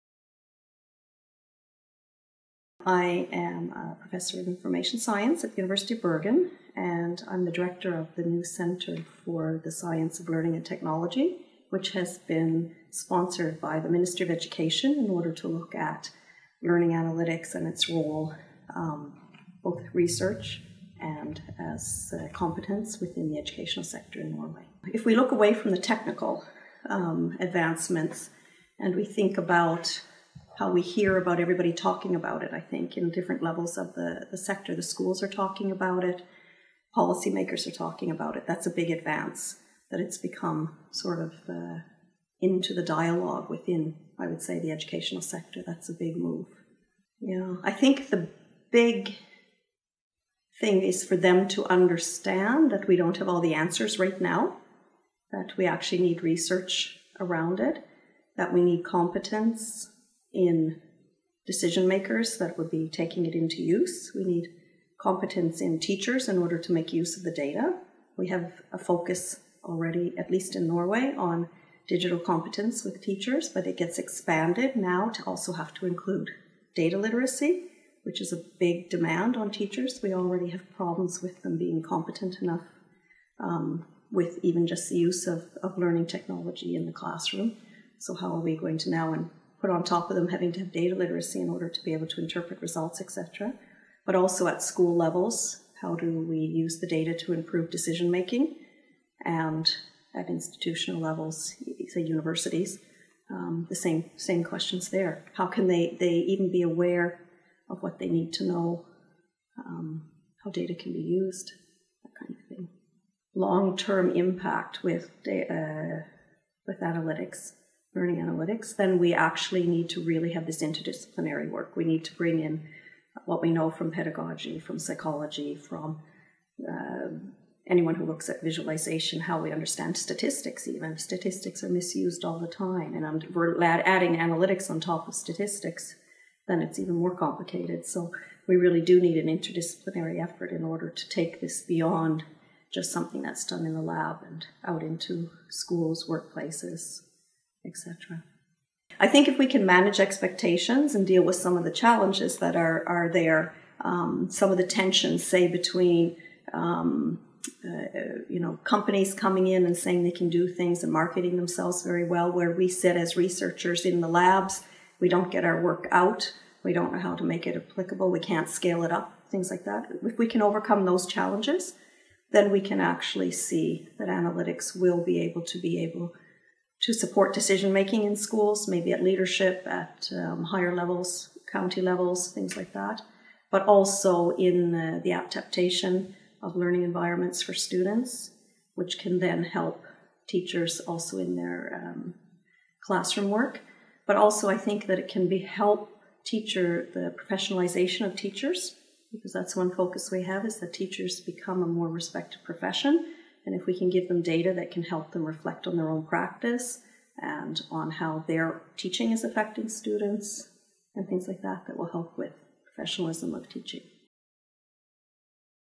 LACE interview with Barbara Wasson
The interview was conducted during a learning analytics experts workshop in Amsterdam in March 2016 by Maren Scheffel and Hendrik Drachsler.